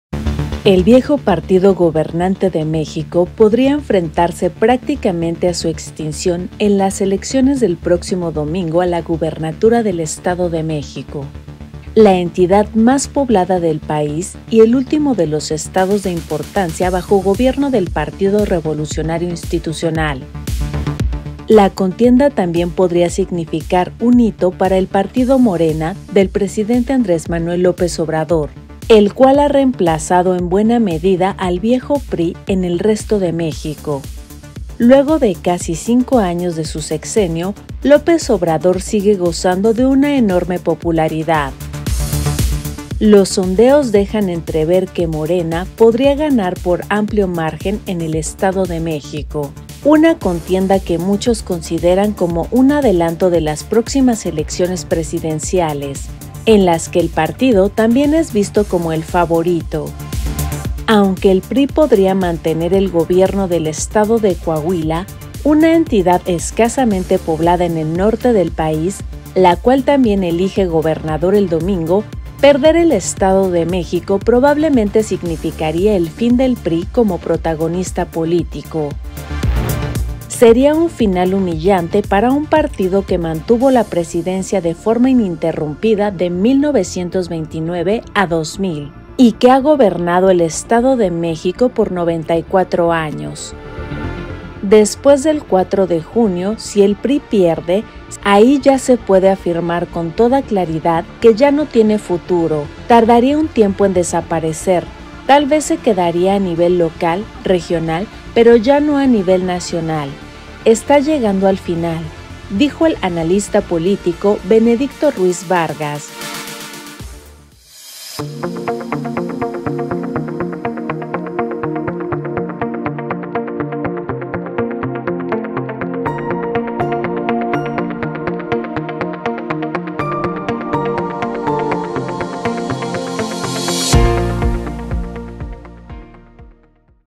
Elecciones en Edomex pueden ser el “principio del fin” del viejo partido mexicano: AP
De acuerdo con AP, perder el Estado de México significaría el fin del PRI como protagonista político a nivel nacional, lo que sería un final humillante para un partido que mantuvo la Presidencia de forma ininterrumpida de 1929 a 2000, y que ha gobernado la entidad por 94 años.